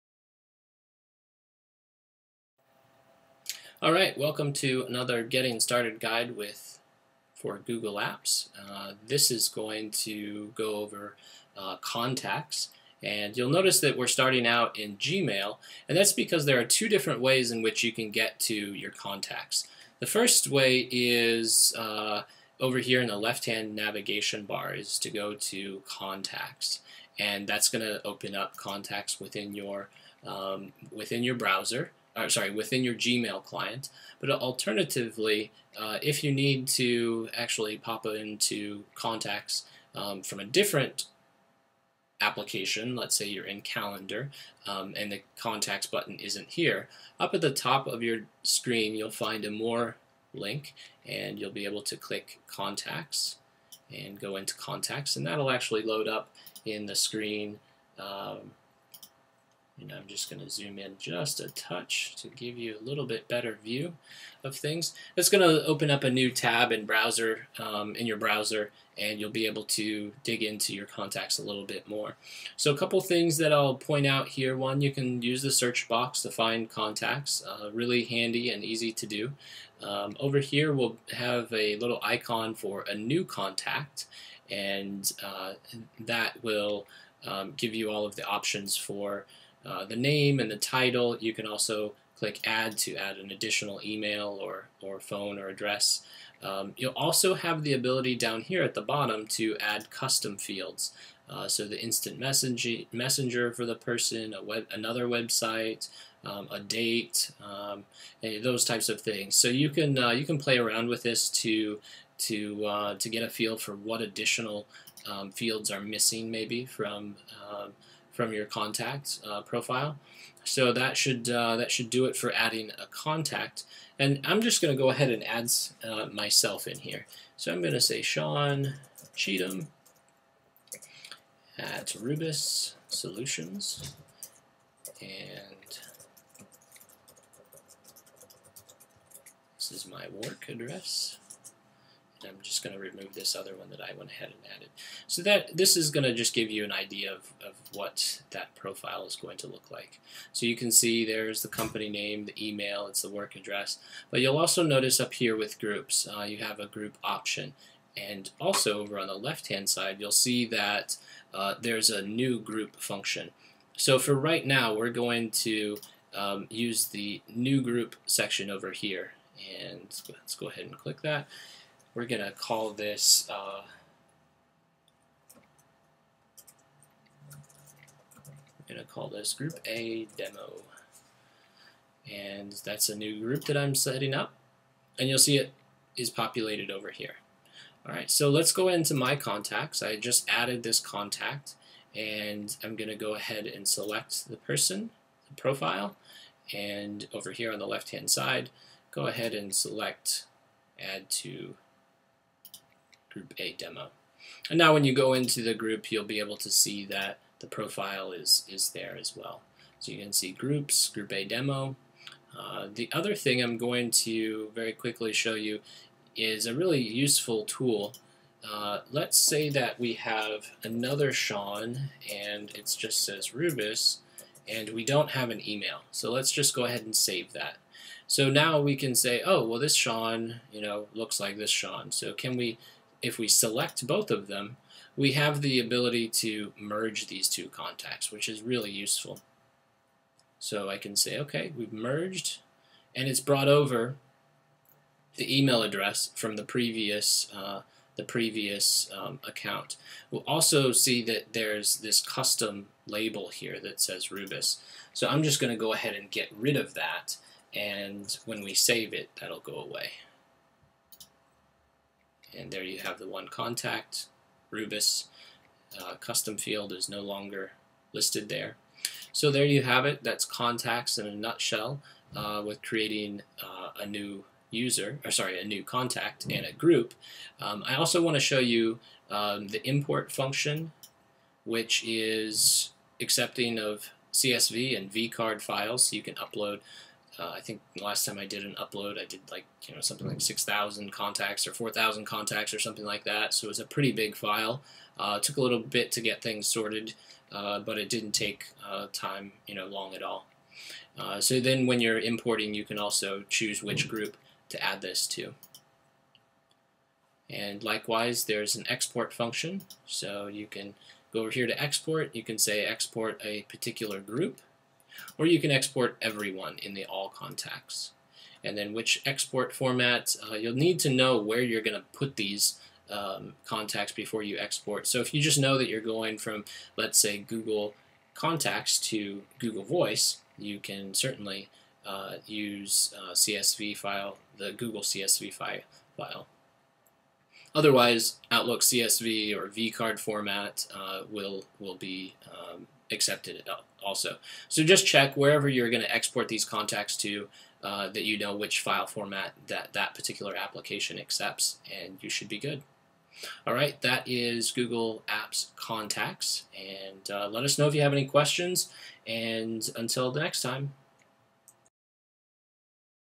Contacts Overview: Google Apps Premier Edition
Take a few minutes to orient yourself with a newly deployed Google Apps Contacts.